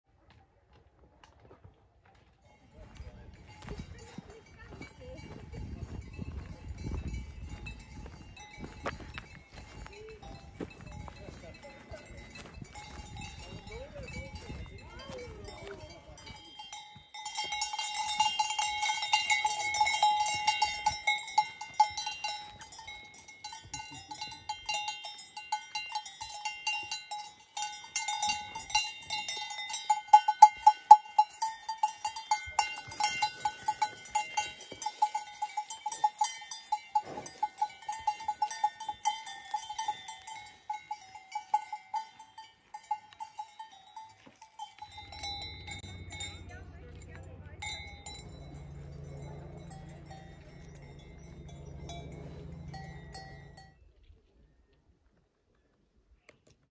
Just Cows In The Alps!
Allgäu 2020
What to say? It is just a 1 min vid about cows in the Alps. Nothing more. Enjoy. Muuuuh